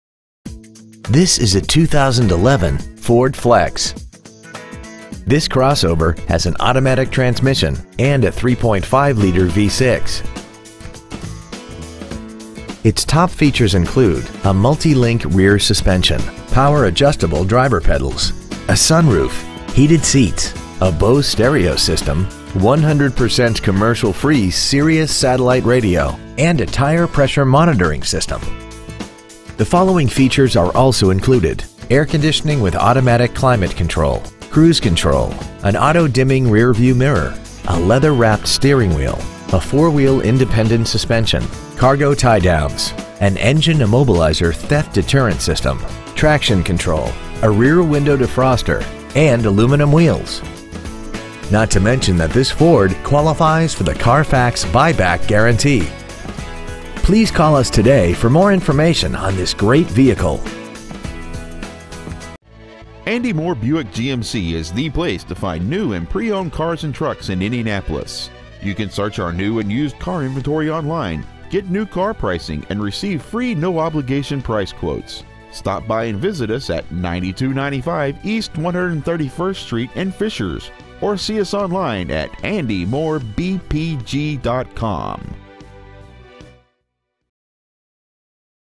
2011 Ford Flex Indianapolis IN 46038
Year : 2011
 Make : Ford
 Model : Flex
 Engine : 3.5L V6 24V MPFI DOHC
 Trans . : 6-Speed Automatic
 Exterior : Tuxedo Black Metallic
 Miles : 78,003
 Interior : Charcoal Black
 Stock : G3176A

 9295 E. 131st Street